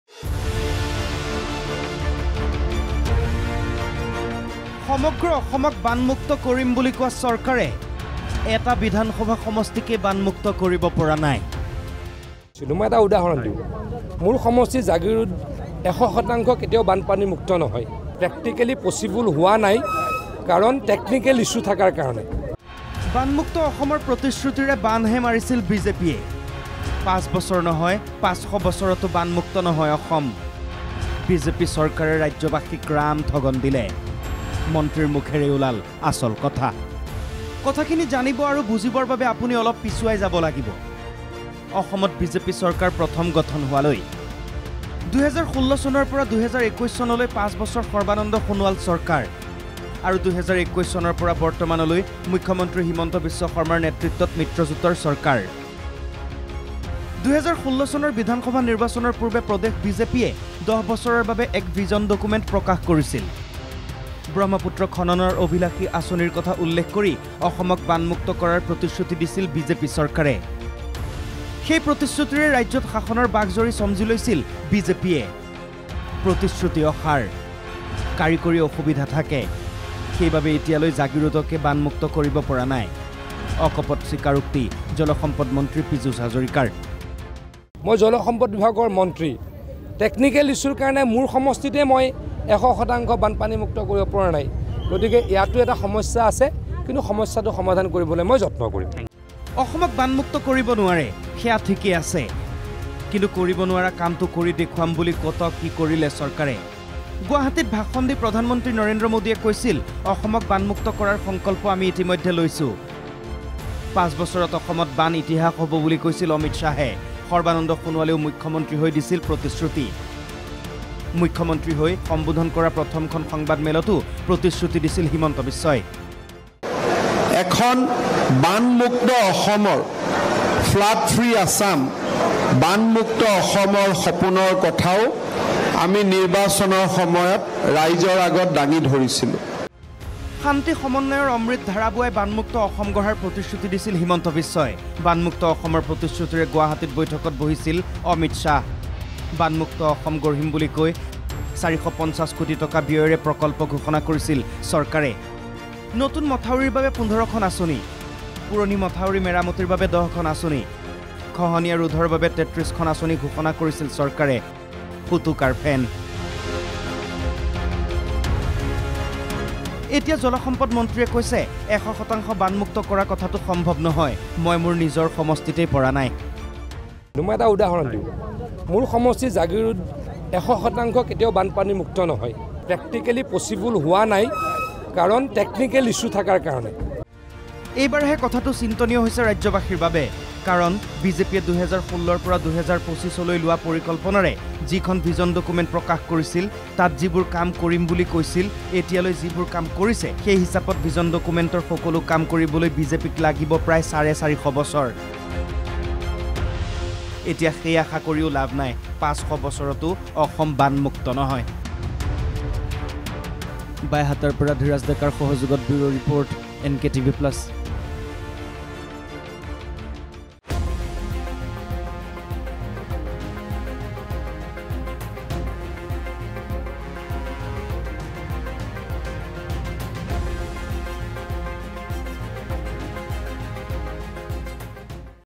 সমগ্ৰ অসমক বানমুক্ত কৰিম বুলি কোৱা চৰকাৰে1
▶️ সমগ্ৰ অসমক বানমুক্ত কৰিম বুলি কোৱা চৰকাৰে
▶️ এটা বিধানসভা সমষ্টিকেই বানমুক্ত কৰিব পৰা নাই
▶️ বানমুক্ত অসমৰ প্ৰতিশ্ৰুতিৰে বাণহে মাৰিছিল বিজেপিয়ে
▶️ ৫ বছৰ নহয়, ৫০০ বছৰতো বানমুক্ত নহয় অসমৰ
▶️ বিজেপি চৰকাৰে ৰাজ্যবাসীক ৰামঠগন দিলে